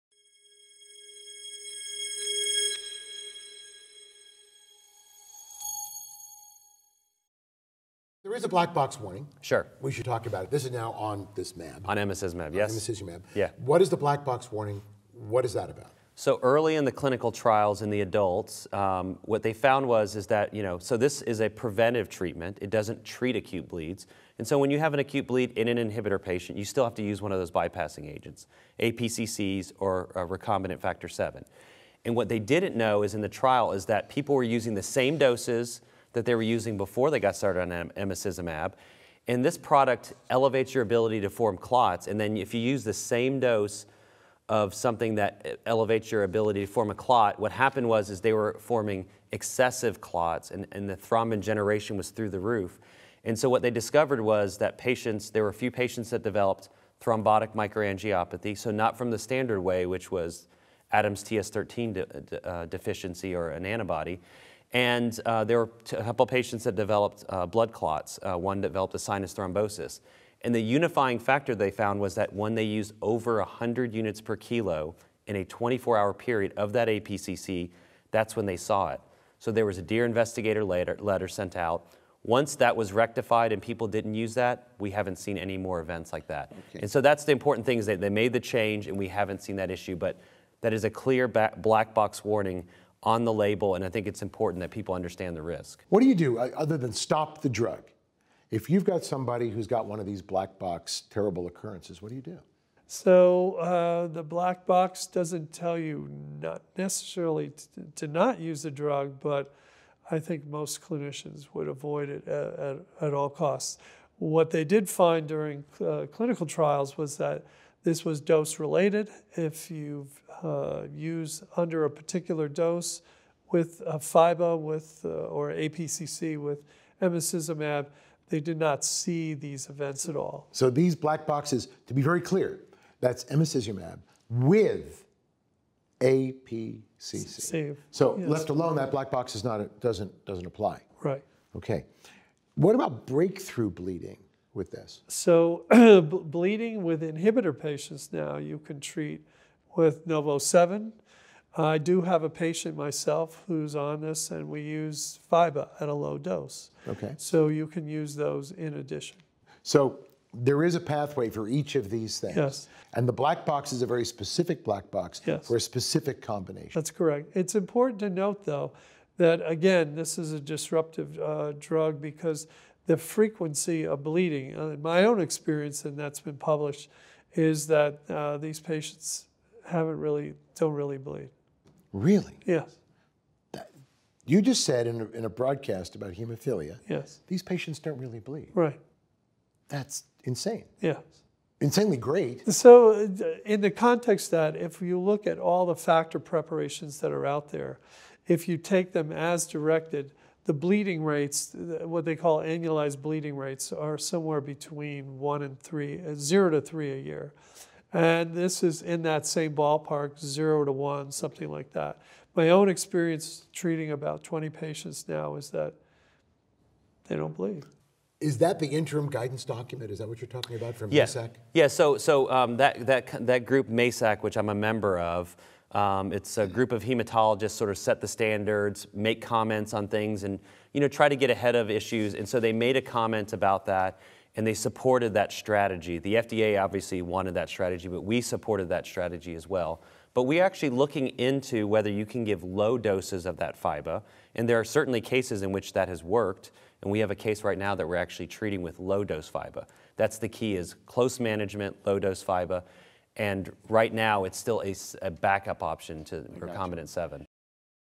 Guidelines for Emicizumab and aPCC Treatment of Hemophilia
Peter L. Salgo, MD; Christopher E. Walsh, MD, PhD; and Robert F. Sidonio Jr, MD, MSc, review the black box warning associated with emicizumab and aPCCs and share the interim MASAC guidelines.